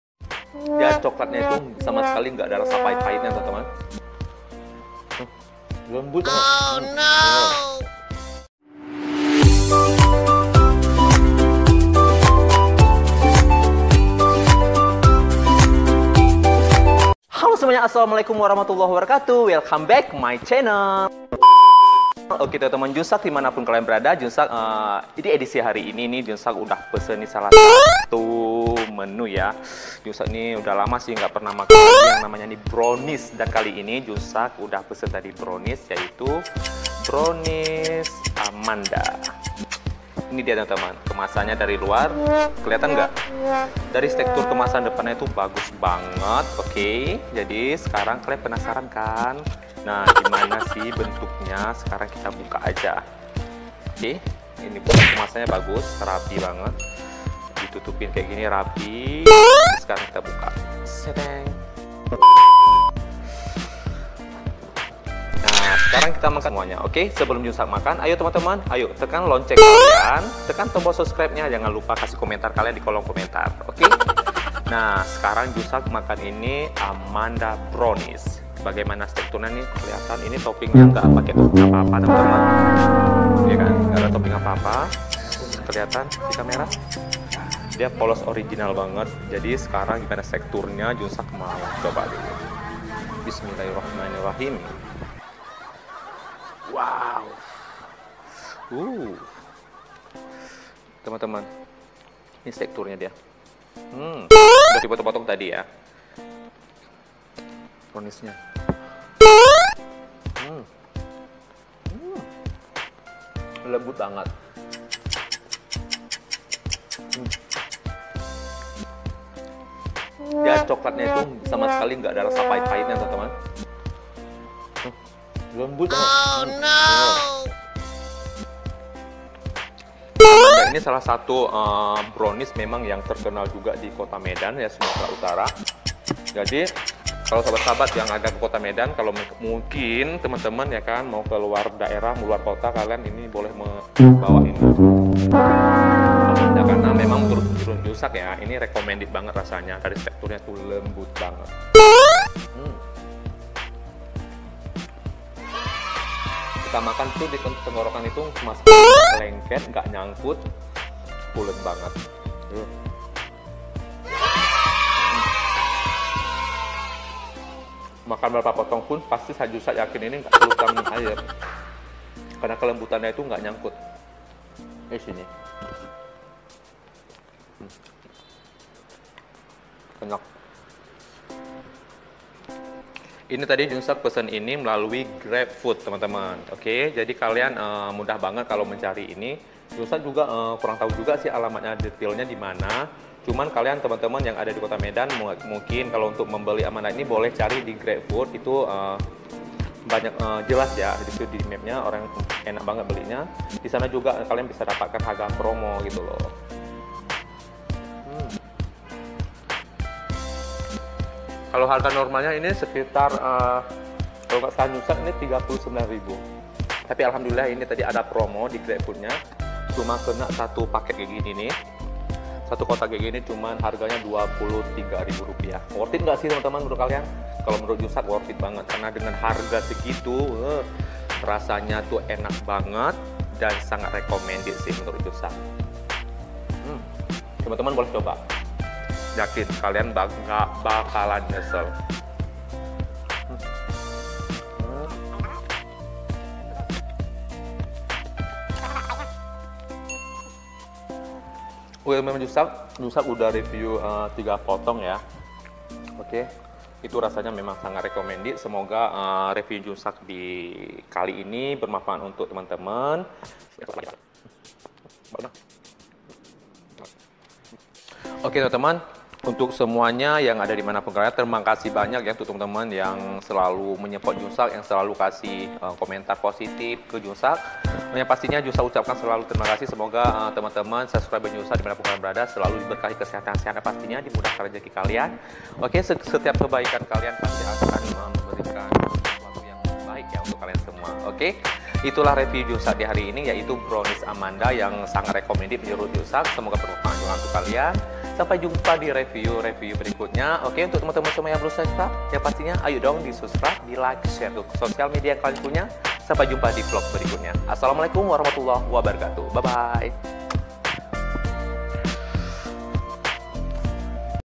BRONIES ORIGINAL AMANDA,REVIEW JUJUR-KULINER MEDAN
BRONIES original merupakan salah satu cake yg jadi favorit junshaq di saat junshaq lagi pingin makan cake,stektur nya lembut dan dengan rasa coklat nya yang pas membuat junshaq ketagihan, kali ini junshaq pesan nya melalui via grab food dan Alhamdulillah dapat promo Lo sahabat junshaq,nonton Vidio review junshaq sampai habis ya,,,

FB. JUNSHAQ JAY
IG. JUNSHAQ